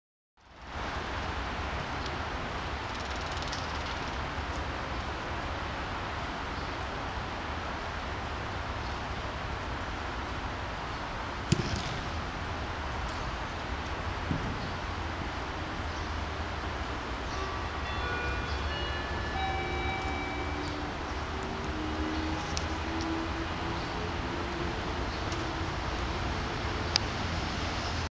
Stade Ernest Wallon
Moi au stade Ernest wallon a toulouse en train de droper !!